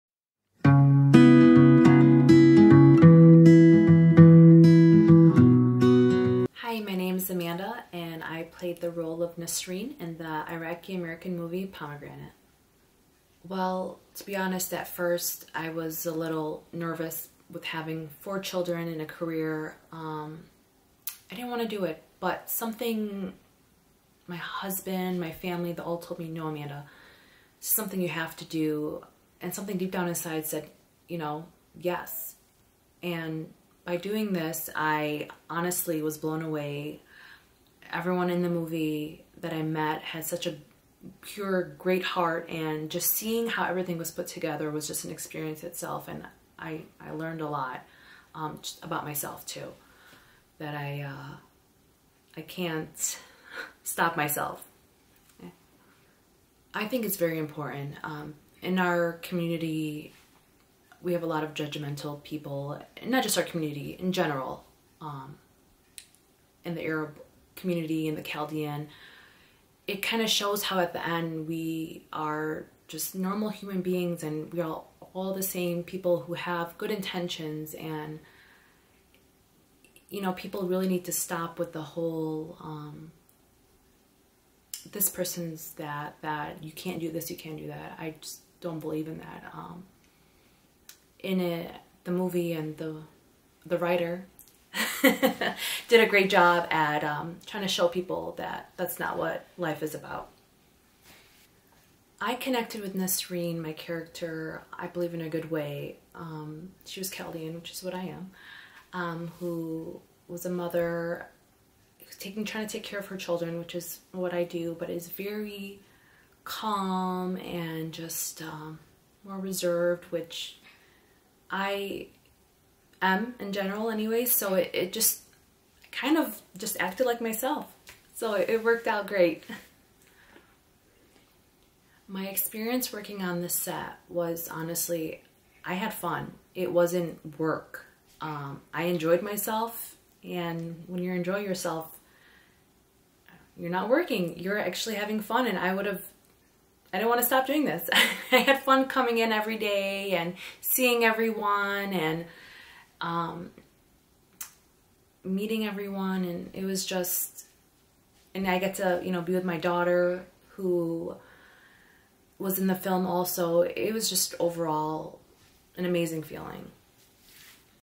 Amanda Kashat (Nisreen) | Pomegranate
Amanda Kashat, who plays Nisreen in the feature film Pomegranate.

Pomegranate book is available in paperback, ebook, and audiobook.